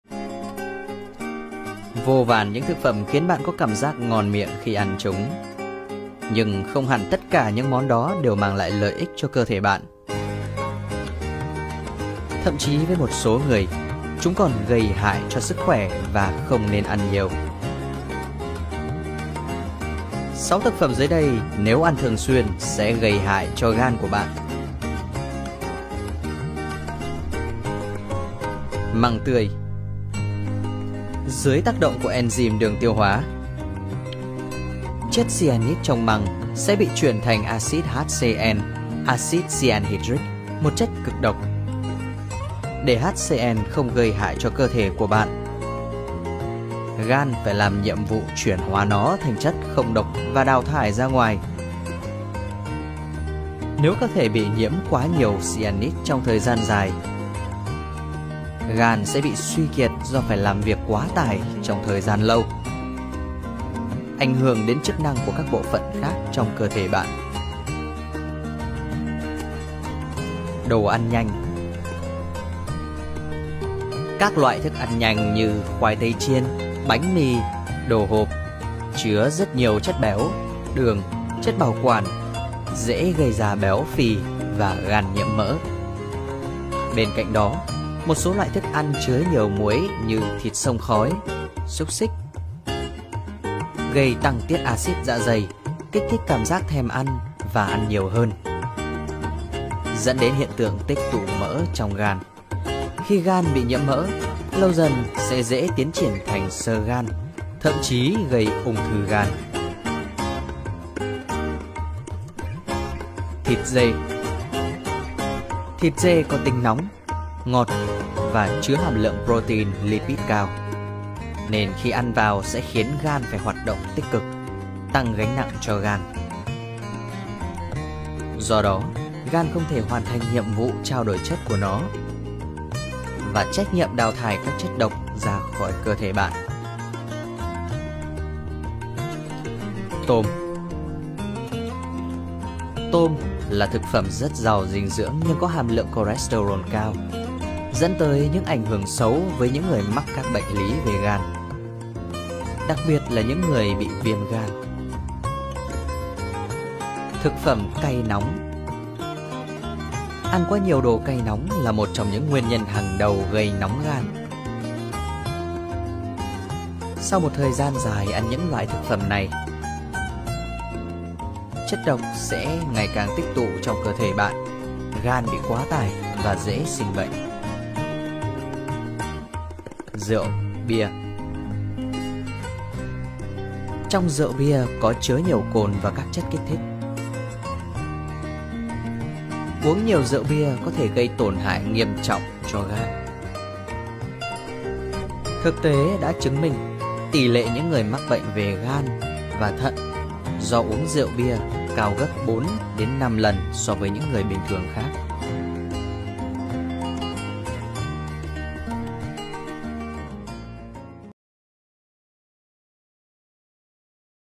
Gan dễ bị tổn thương do thường xuyên ăn 6 loại thực phẩm này
Gan là nội tạng lớn nhất trong cơ thể. Gan thực hiện một số chức năng, bao gồm xử lý mọi thứ mà chúng ta ăn và uống vào, lọc các chất độc hại khỏi máu, điều tiết quá trình chuyển hóa, tổng hợp protein, trữ vitamin và sắt cũng như sản sinh ra mật.
Sự suy giảm hoặc gián đoạn trong bất kỳ quá trình nào do các bệnh về gan có thể dẫn đến các biến chứng nghiêm trọng, suy gan và thậm chí là tử vong nếu bệnh gan tiến triển tới giai đoạn cuối.
Sau đây là 6 loại thực phẩm nếu bạn ăn thường xuyên sẽ khiến gan bị tổn thương.